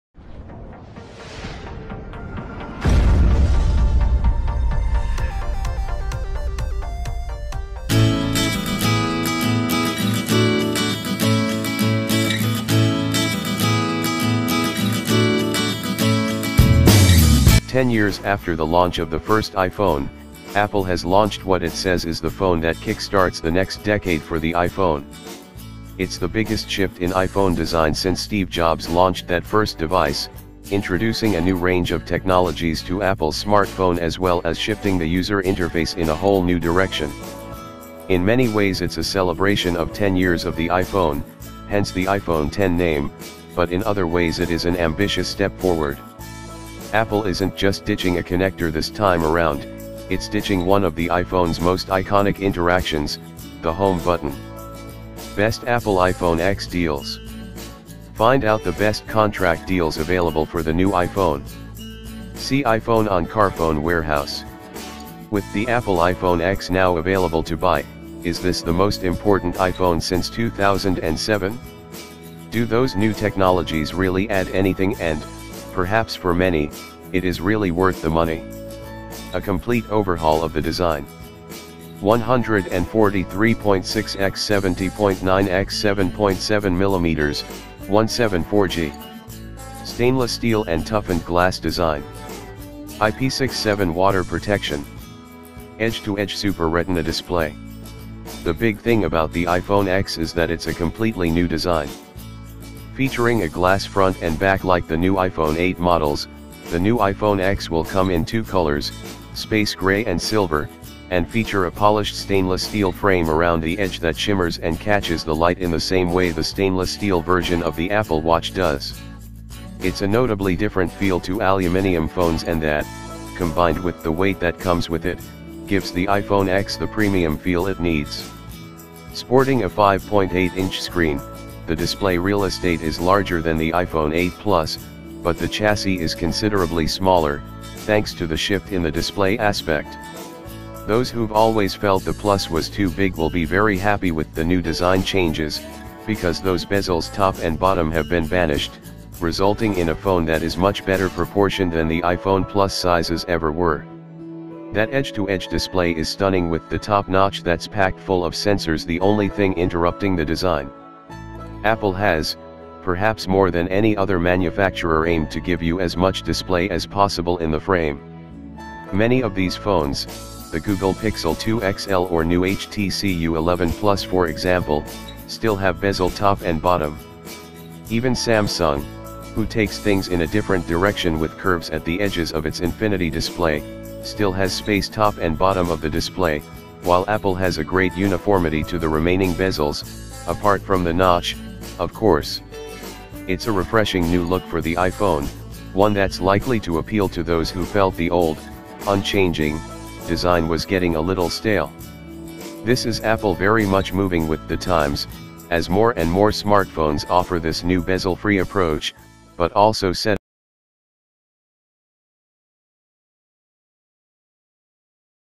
SEE NOW apple iphone x presentation
Ten years after the launch of the first iPhone, Apple has launched what it says is the phone that kickstarts the next decade for the iPhone. It's the biggest shift in iPhone design since Steve Jobs launched that first device, introducing a new range of technologies to Apple's smartphone as well as shifting the user interface in a whole new direction.

In many ways it's a celebration of 10 years of the iPhone - hence the iPhone 10 name - but in other ways it is an ambitious step forward. Apple isn't just ditching a connector this time around, it's ditching one of the iPhone's most iconic interactions: the home button.

Credit by :

Reference :
Images :